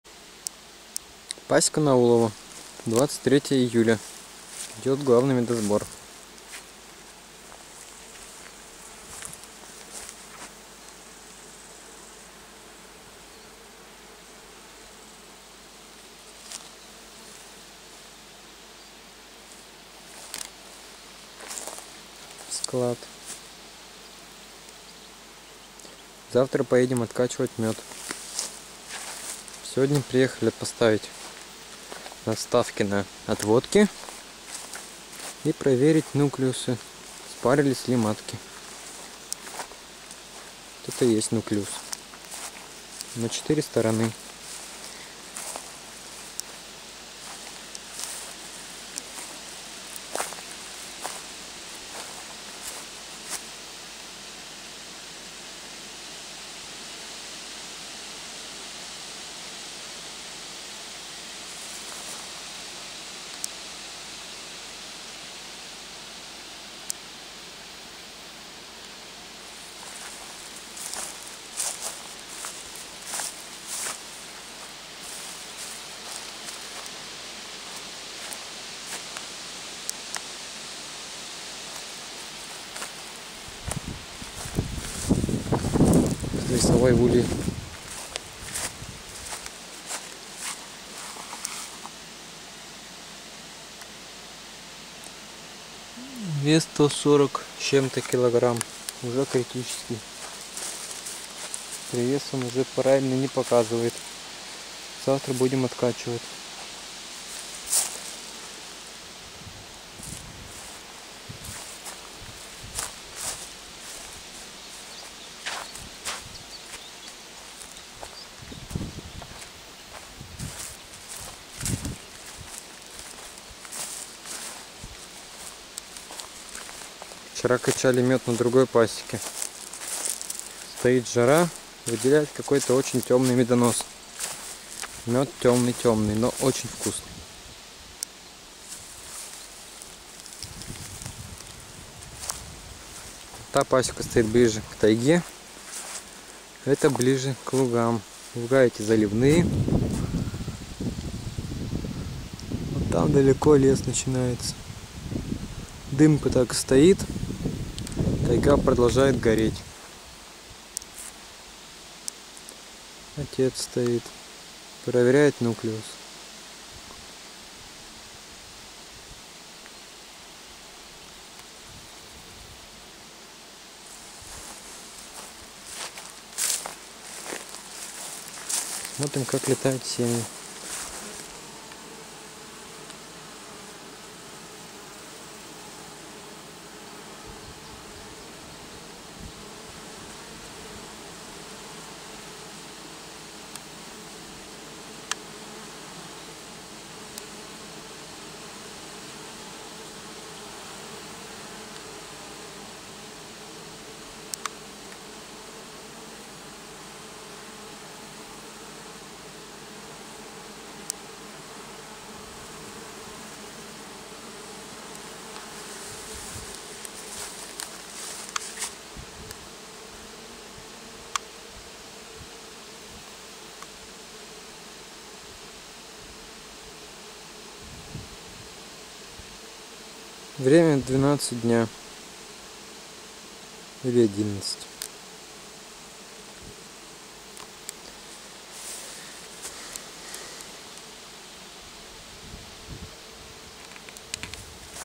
Прогулка по пасеке Старчевских. Улово, 23 июля 2012
Прогулка по пасеке Старчевских. Боготольский район. Красноярский край. Кочевая пасека. 23 июля 2012 год. Через неделю закончится медосбор.
Подробнее о нас на 24medok.ru